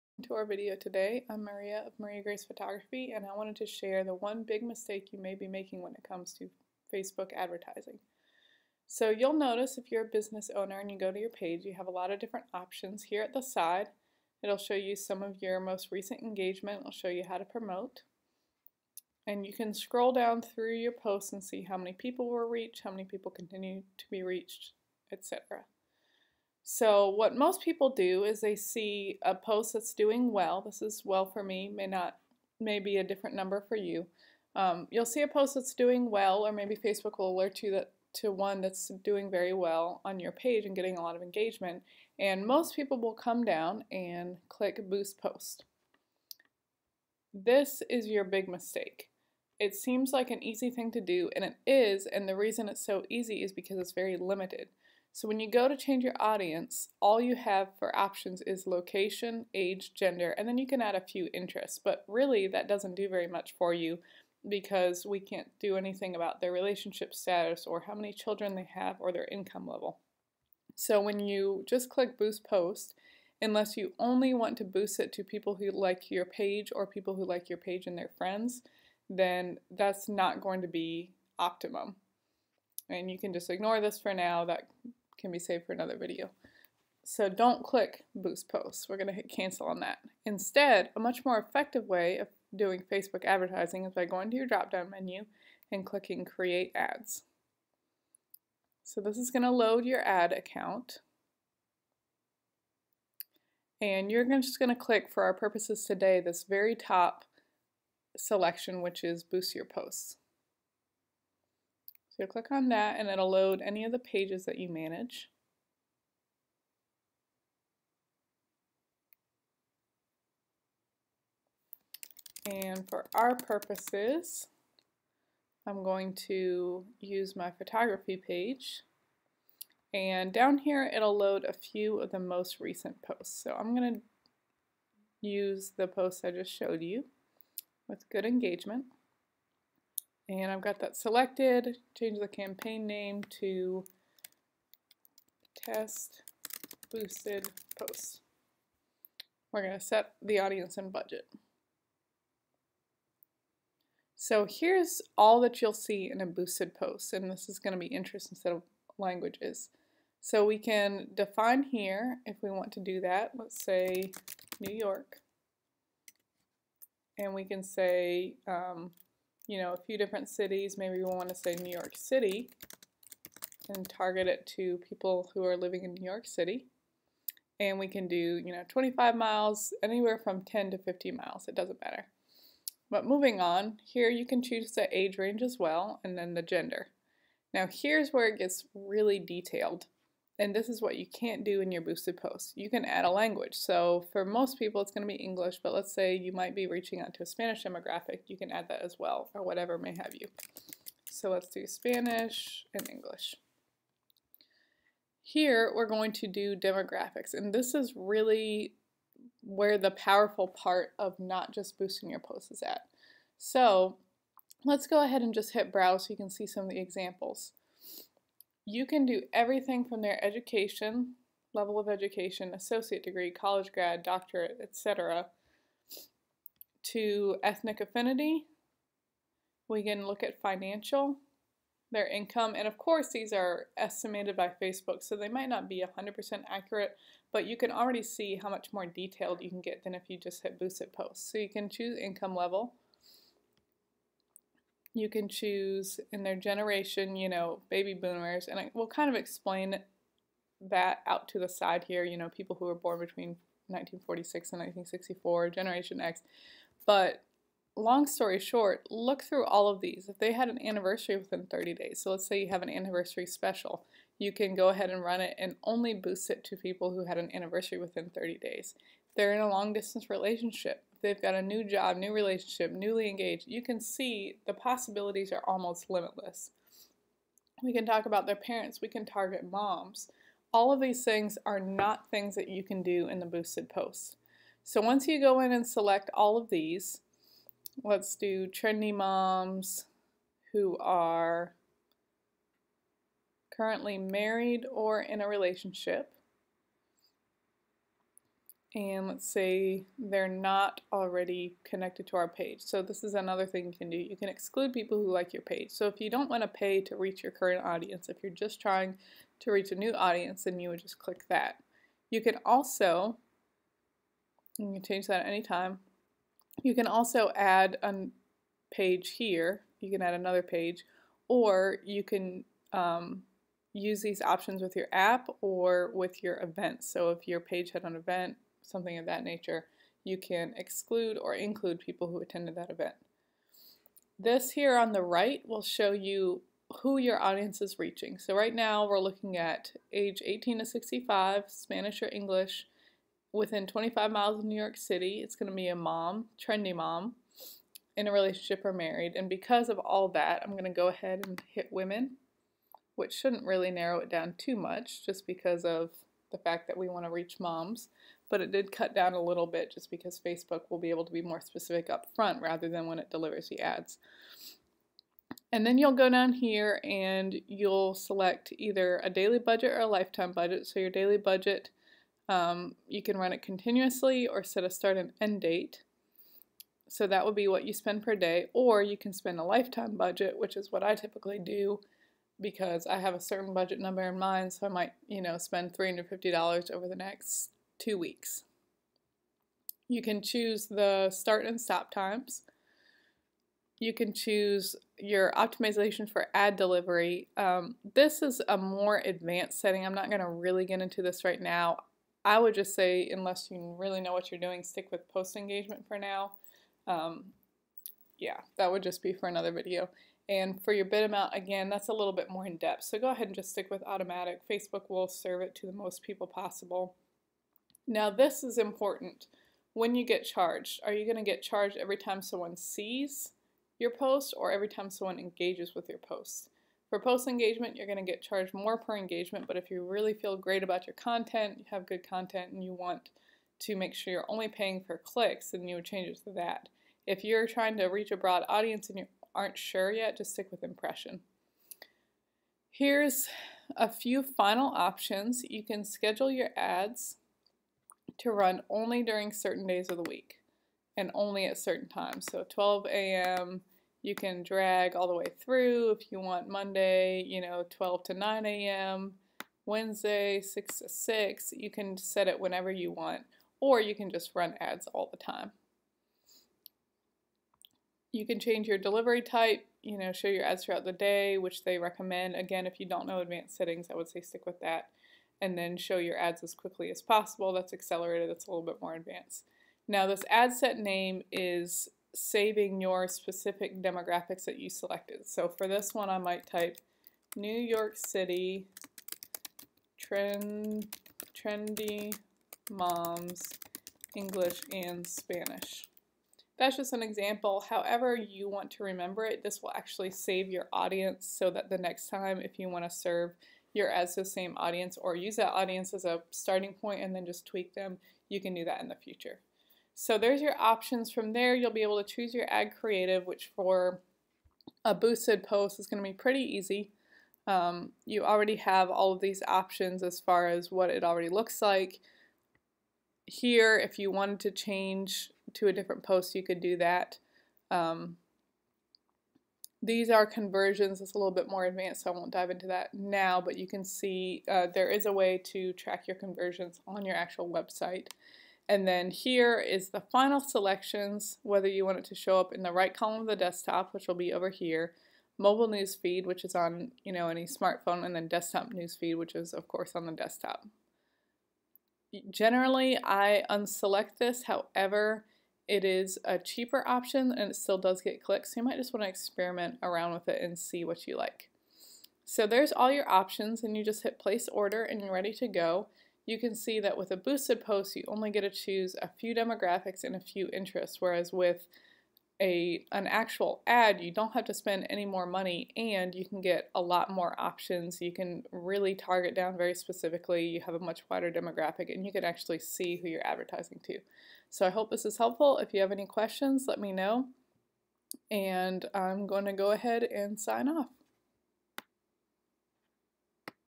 The #1 Mistake You're Making with Facebook Ads
Many people have served a Facebook ad at some point, but most people are doing it all wrong. In this video, find out the number one mistake people are making with Facebook advertising, and how you can serve your ads to your audience.